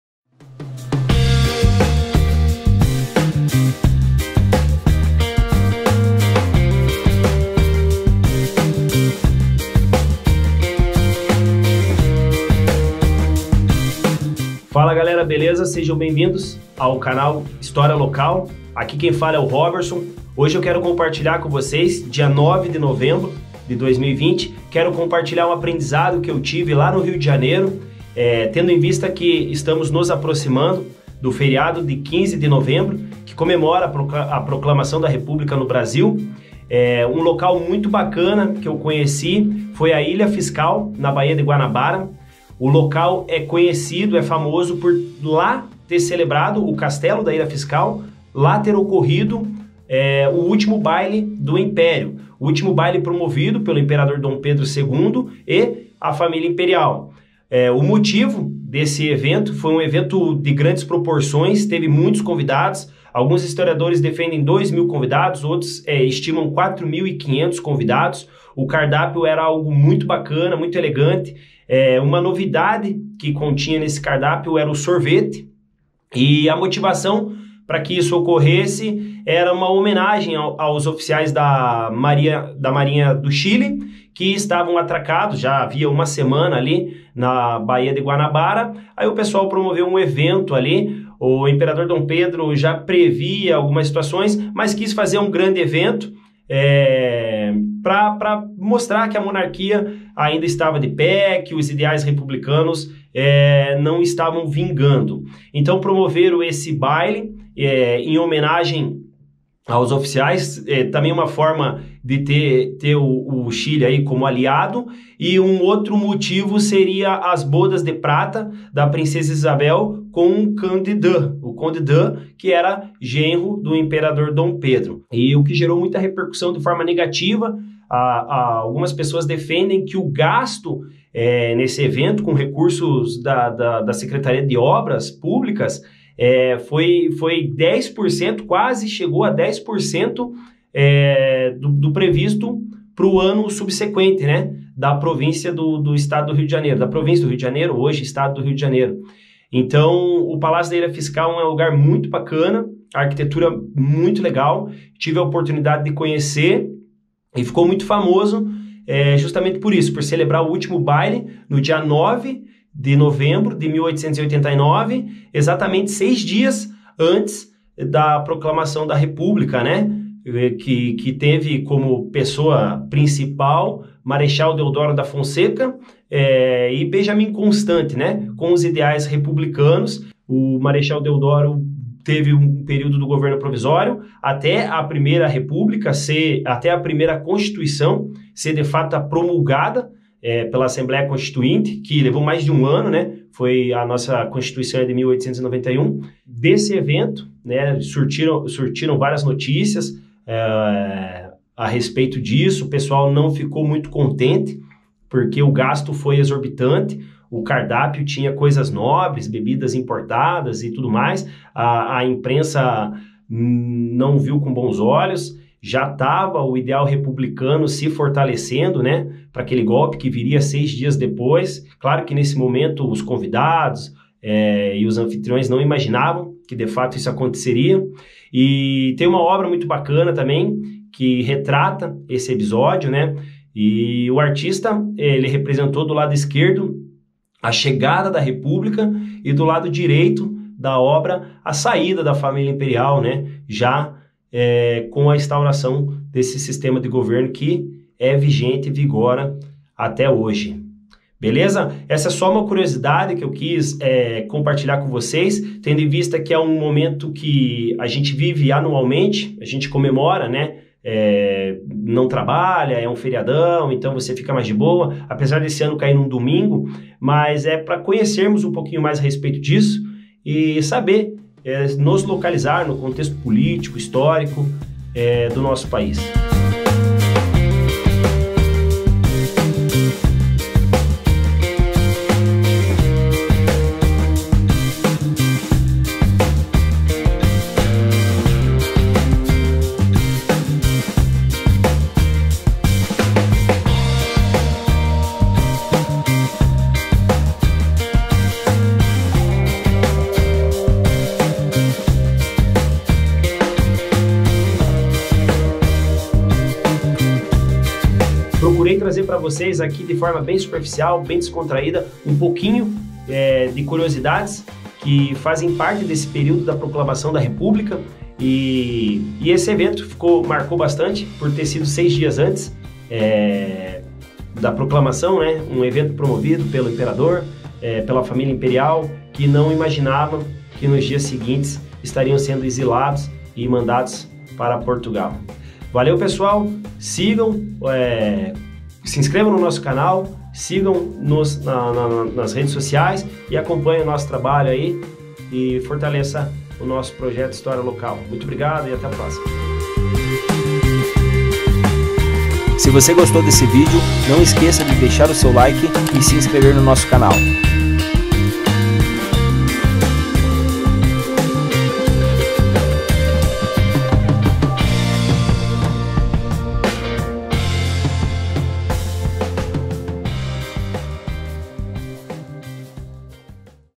O ÚLTIMO BAILE DO IMPÉRIO - ILHA FISCAL, RIO DE JANEIRO (1889).
Uma breve abordagem sobre o último baile promovido pela família imperial no Brasil, no dia 9 de novembro de 1889, seis dias antes de ser proclamada a República no país.

Dom Pedro recusou sufocar o golpe, liderado principalmente por Marechal Deodoro da Fonseca, seguiu para o exílio em Portugal com todos os integrantes da Família Real no dia 16 de novembro de 1889.